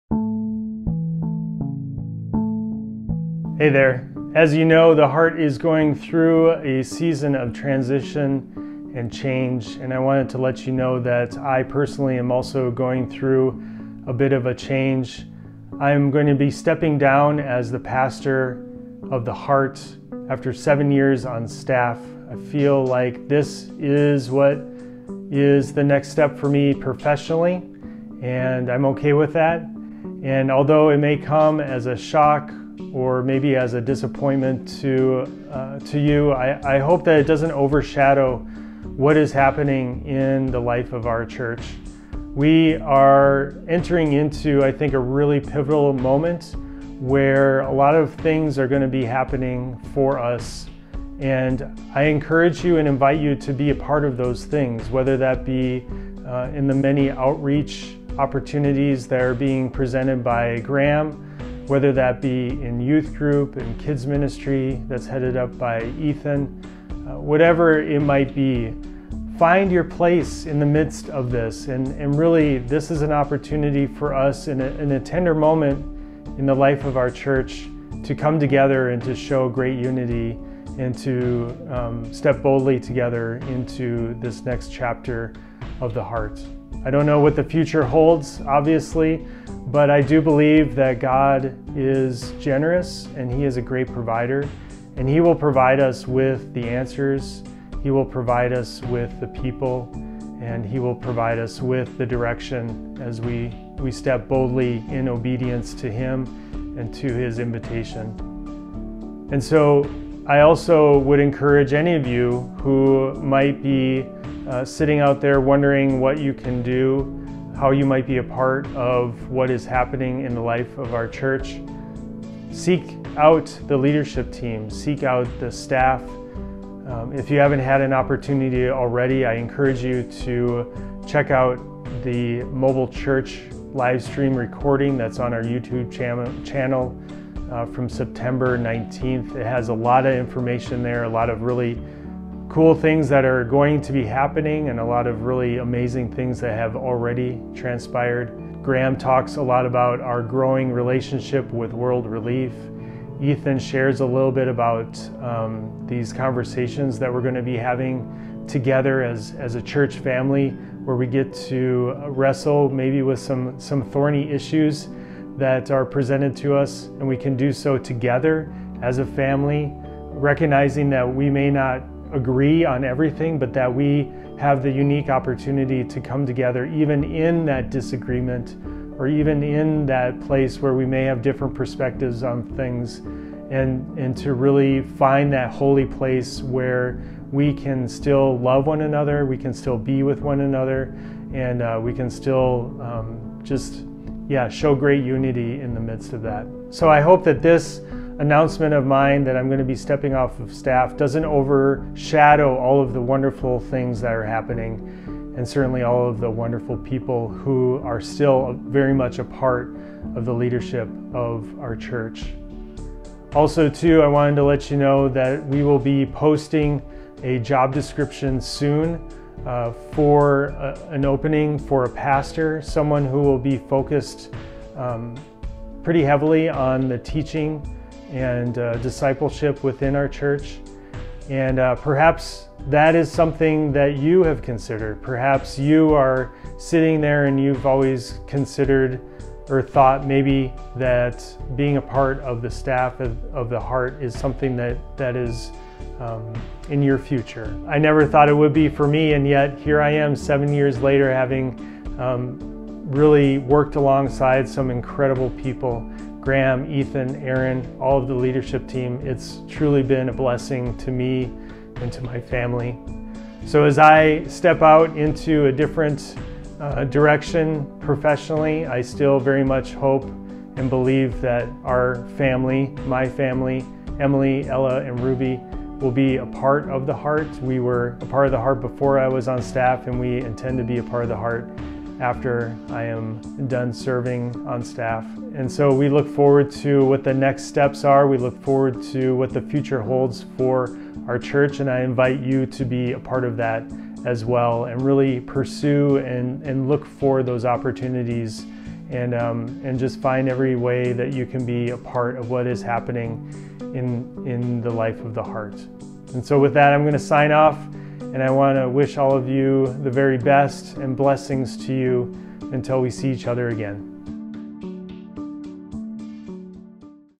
An Important Announcement from Pastor Josh | theHeart Boone
Pastor Josh breaks some important news. He also looks ahead at the conversations on our church future and encourages you to get involved.